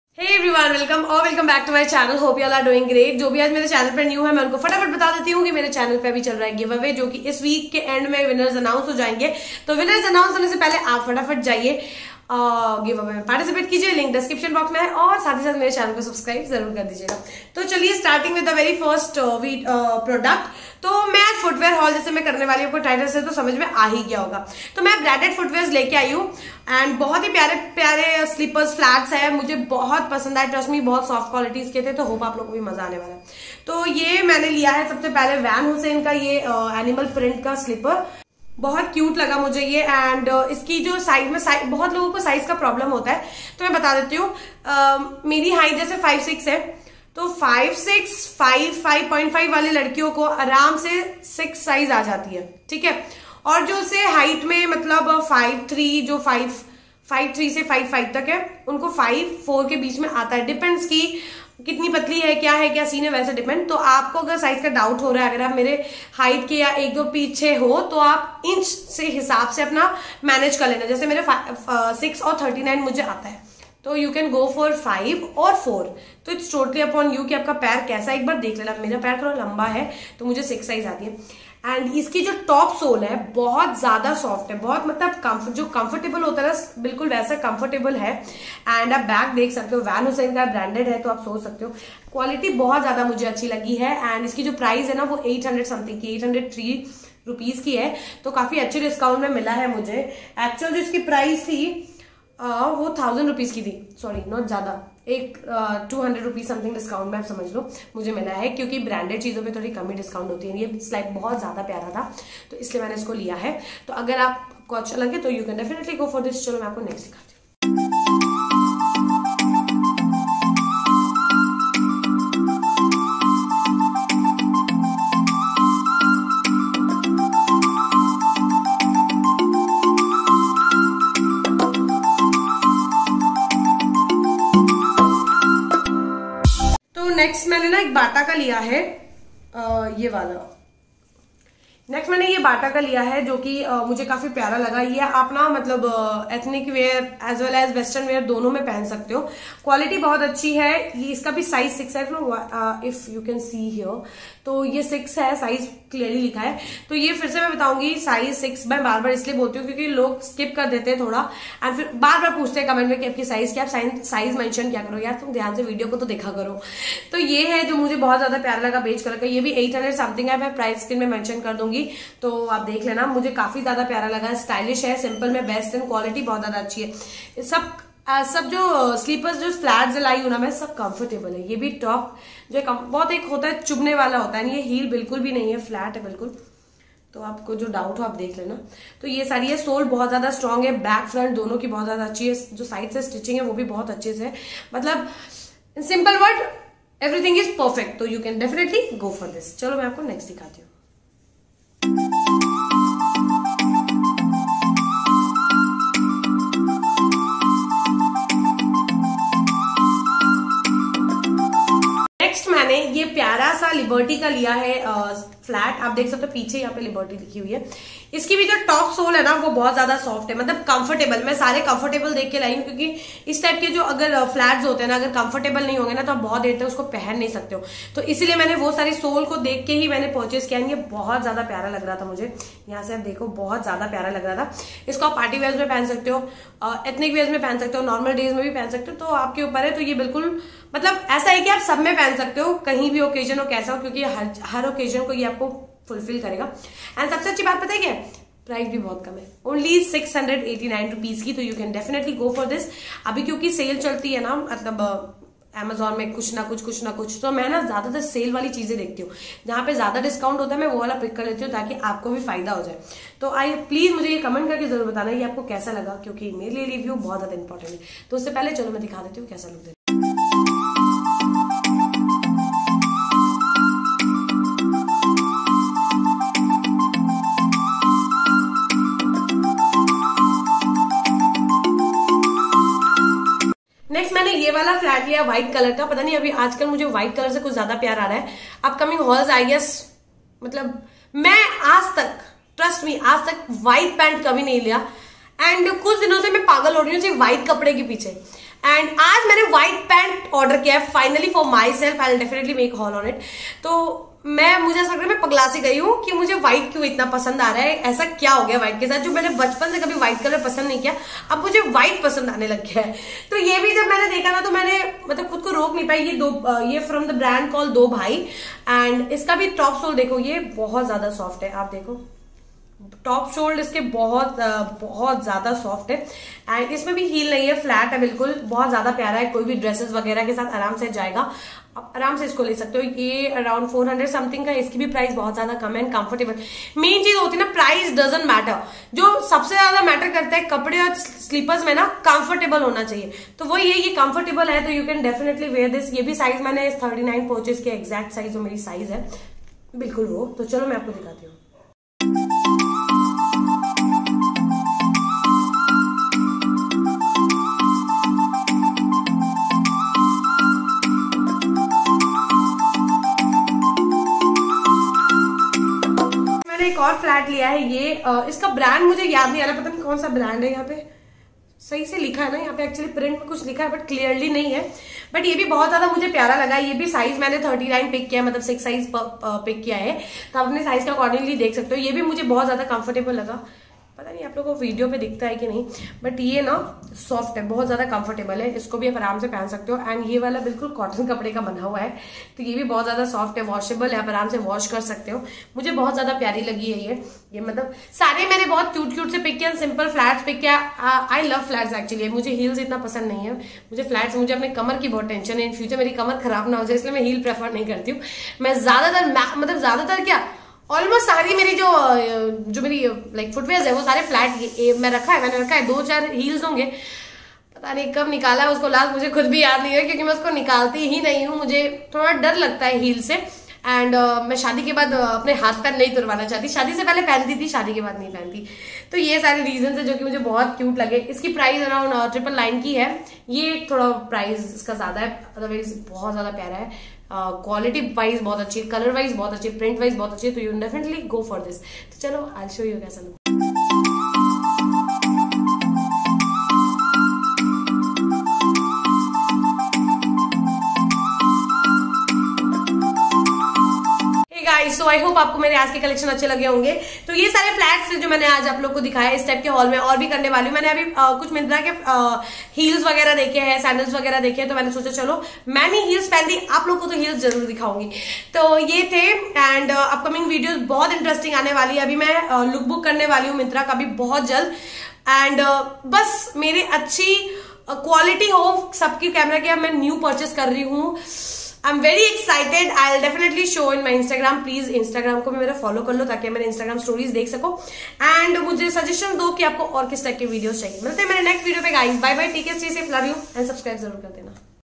*Branded*Amazon Footwear Haul Starts Rs 399 | Flats, branded Footwear haul | Amazon Sale 80% Off
Buying Links :

1. Shoetopia Womens Stylish Trending Flats

2. Limited-time deal: YOGA BUTTERFLY| Super Soft | Comfort | Cushion | Bounce Back | Durable | Handcrafted Upper | Outdoor | Sandals for Women

3. BATA womens Palma Thong Fashion Slippers

4. Liberty womens Eliza-5 Slipper

5. Van Heusen girls Sliders